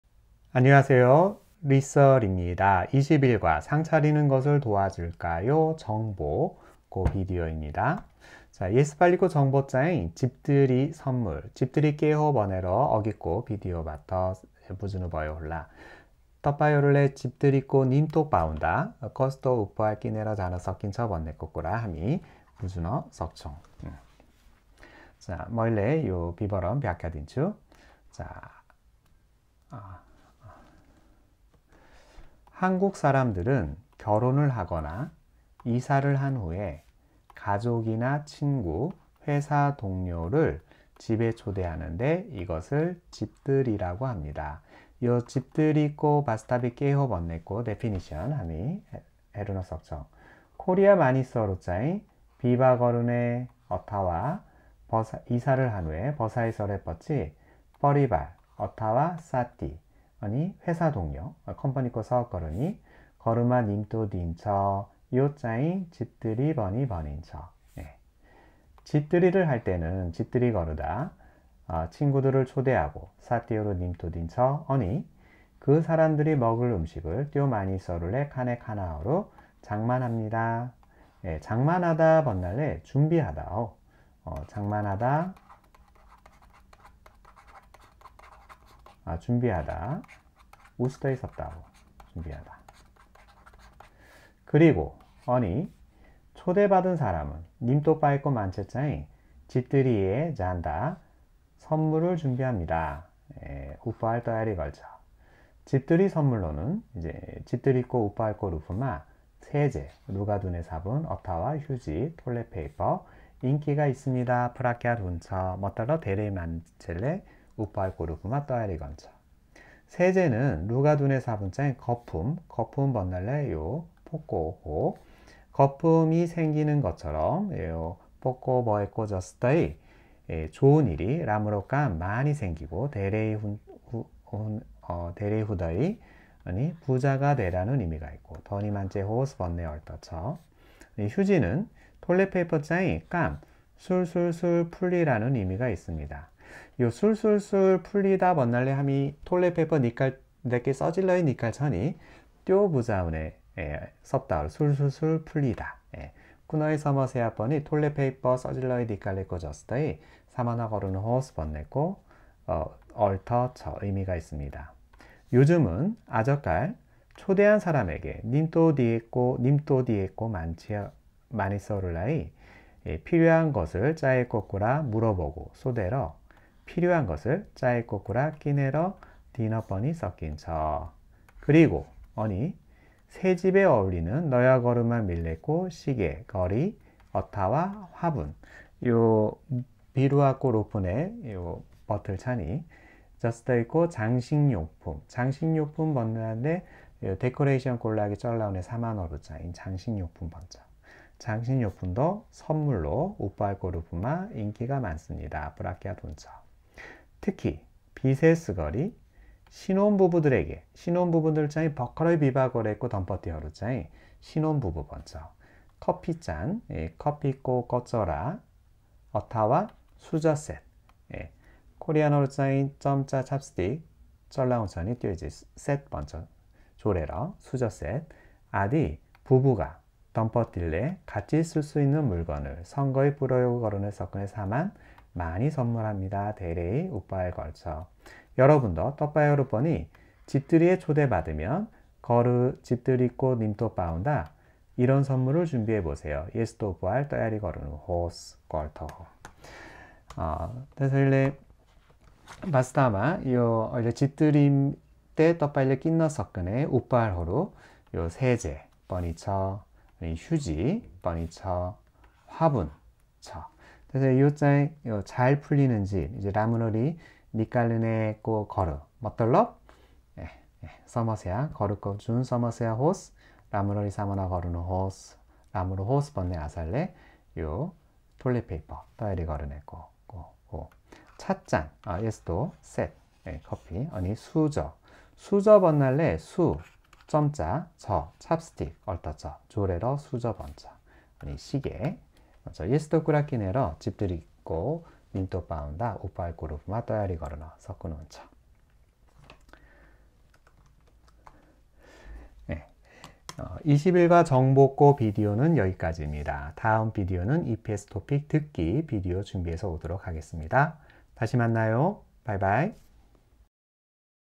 leesir2108 [EPS-TOPIK] Korean Text Book Chapter 21 Part 8 - 문화/정보 (Culture/Information)
21. 상 차리는 것을 도와줄까요? ( leesir2108 )
Information: 집들이 선물
한국 사람들은 결혼을 하거나 이사를 한 후에 가족이나 친구, 회사 동료를 집에 초대하는데 이것을 ‘집들이’라고 합니다. 집들이를 할 때는 친구들을 초대하고 그 사람들이 먹을 음식을 장만합니다. 그리고 초대 받은 사람은 집들이에 갈 때 선물을 준비합니다. 집들이 선물로는 세제나 휴지가 인기가 있습니다. 세제는 거품 (फोको) 이 생기는 것처럼 좋은 일이 많이 생기고 부자가 되라는 의미가 있고, 휴지는 일이 술술 풀리라는 의미가 있습니다. 요즘은 초대한 사람에게 필요한 것을 물어보고 필요한 것을 사 주기도 합니다. 그리고 새집에 어울리는 시계나 화분 같은 장식 용품도 선물로 인기가 많습니다. 특히 신혼부부들에게는 커피 잔이나 수저 세트 등 부부가 같이 쓸 수 있는 물건을 많이 선물합니다. 여러분도 집들이에 초대 받으면 이런 선물을 준비해 보세요.